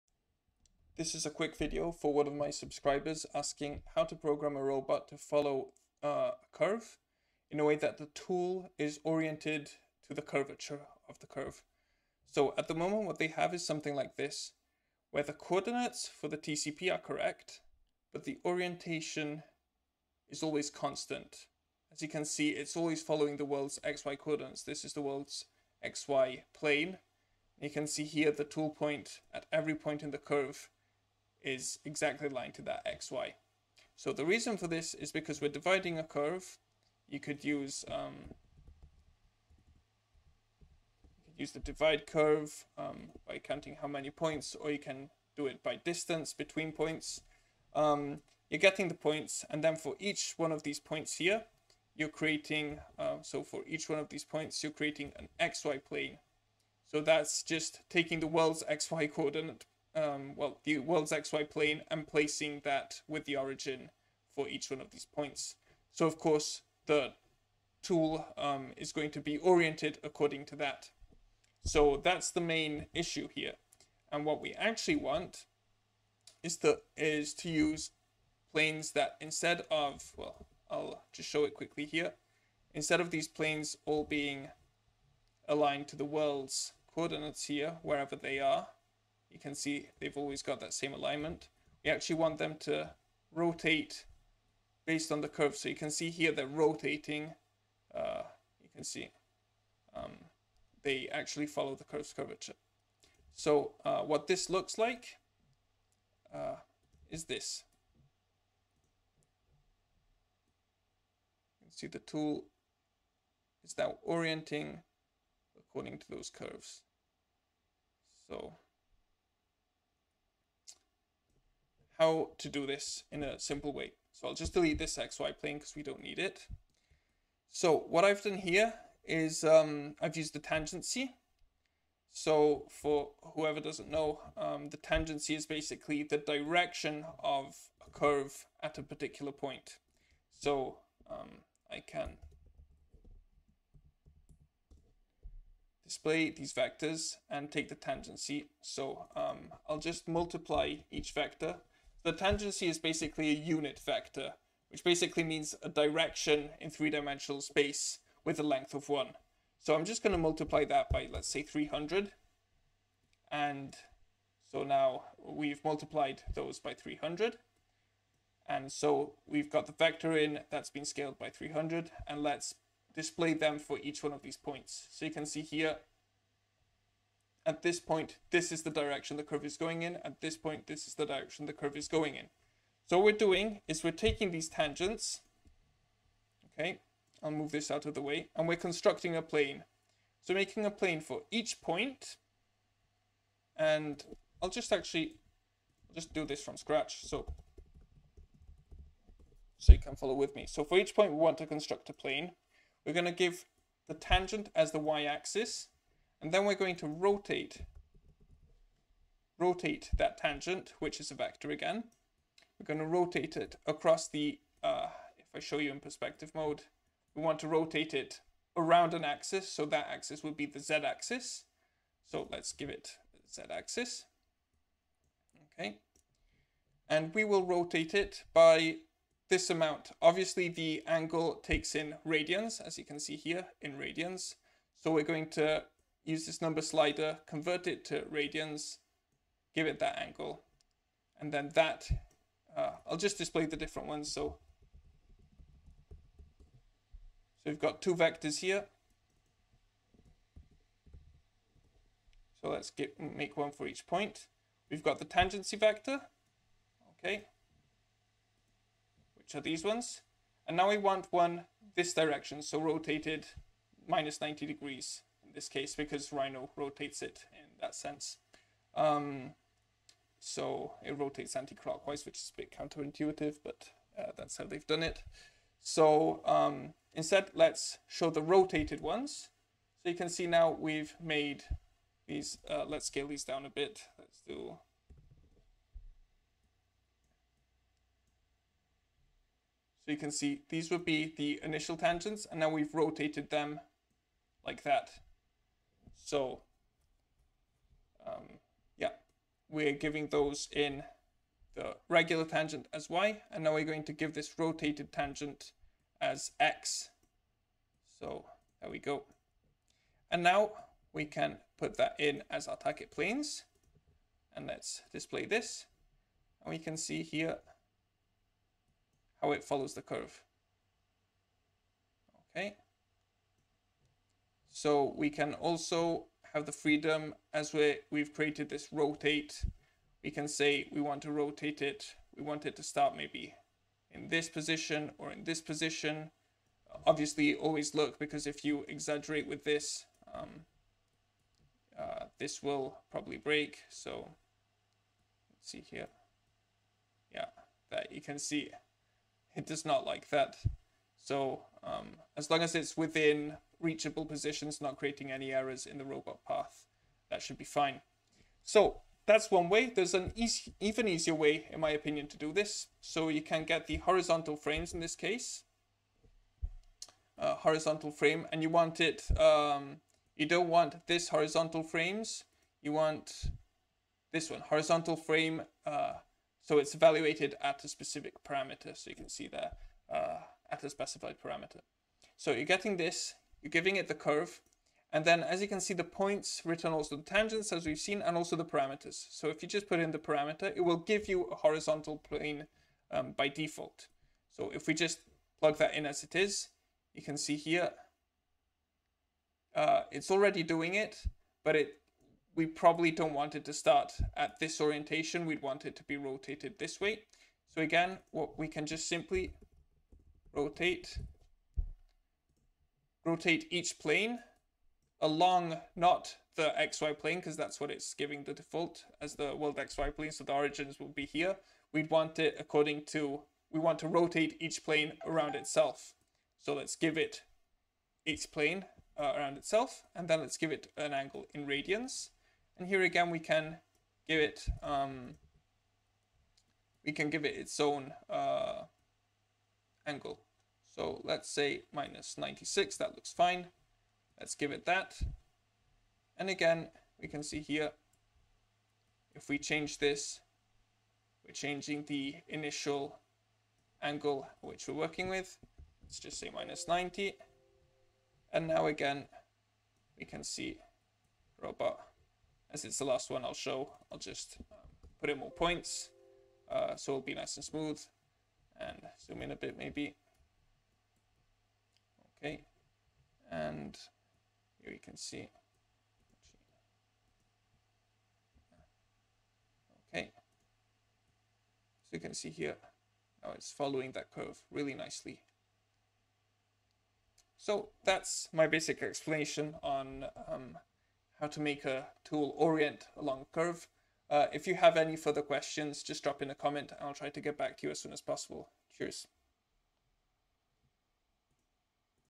How To Orient A Robot Tool Along a Curve (KUKA|prc)
Short video for one of my subscribers showing how to orient a robot tool to a curve as it moves through it.